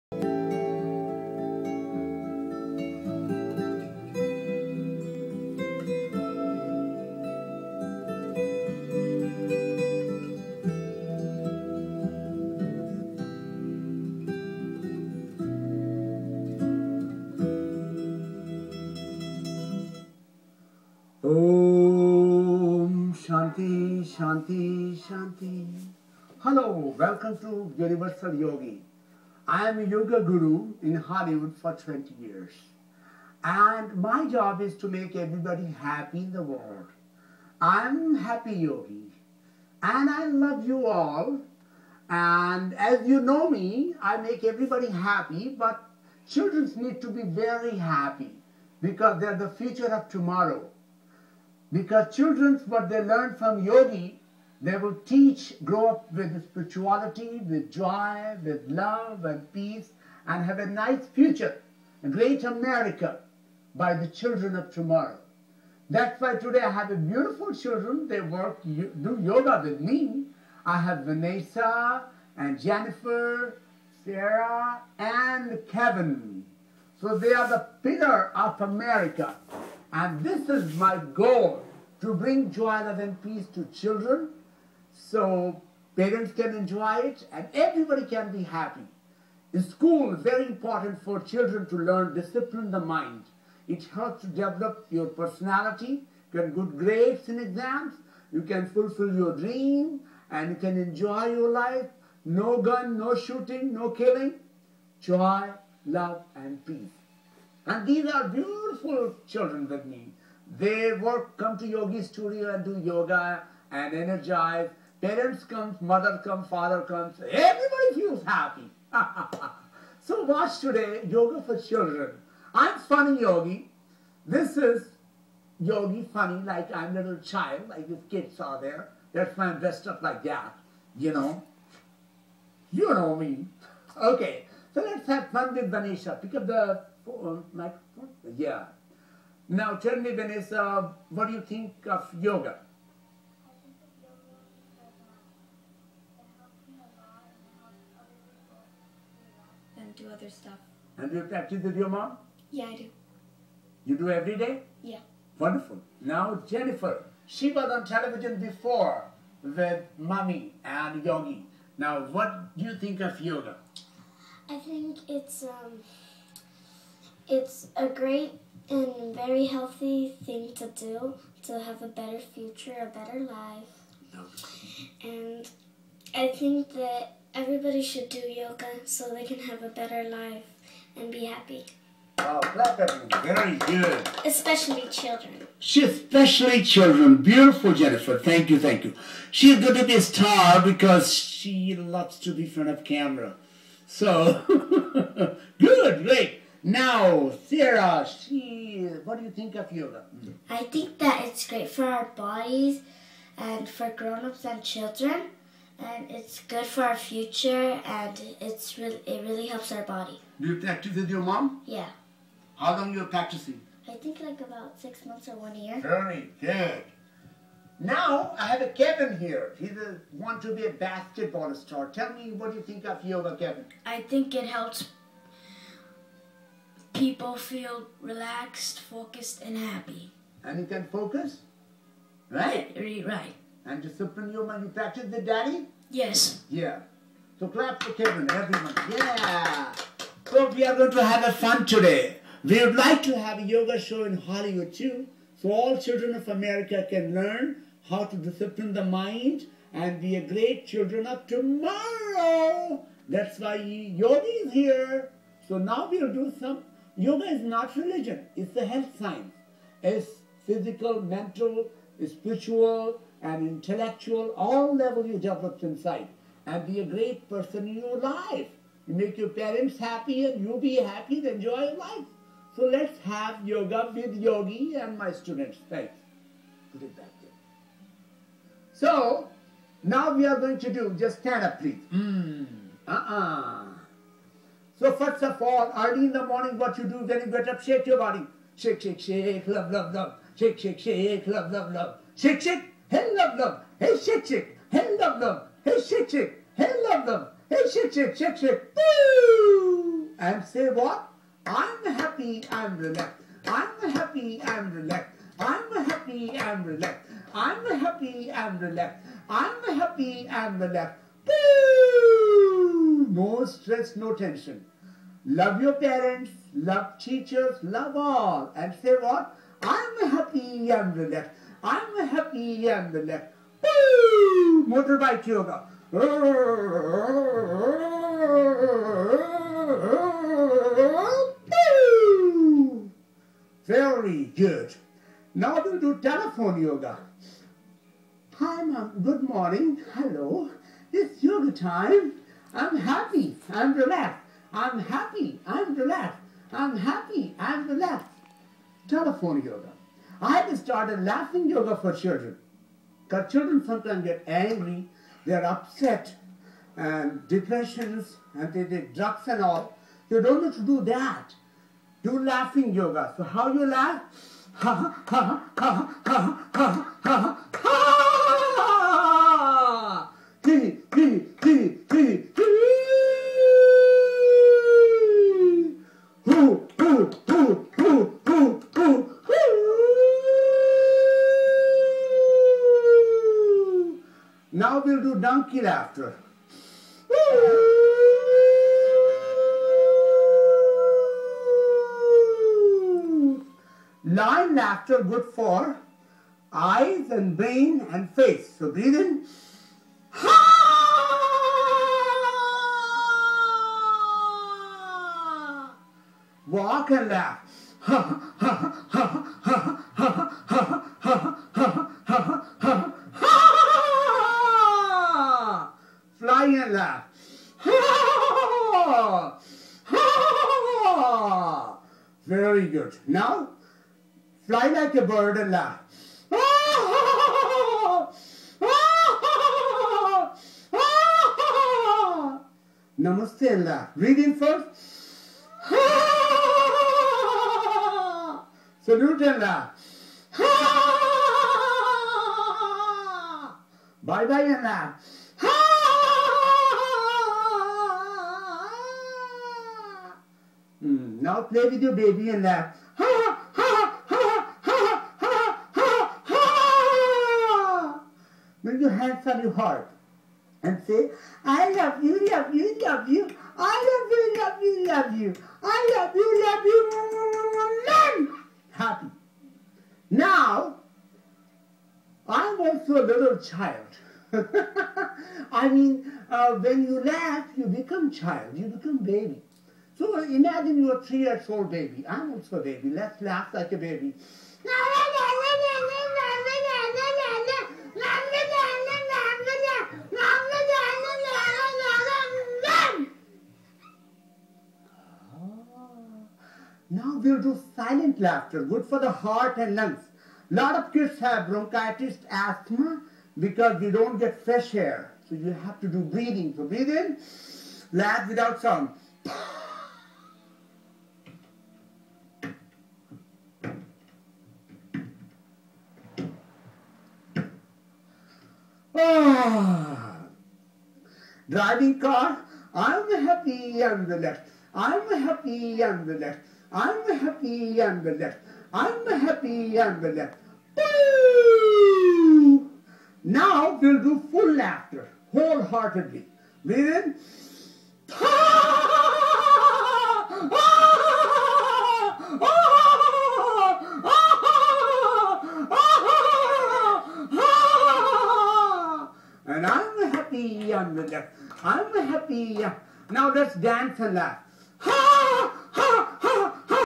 Happy yoga for children with Yogi Ramesh
Happy yoga is good for anger,anxiety,depression and stress.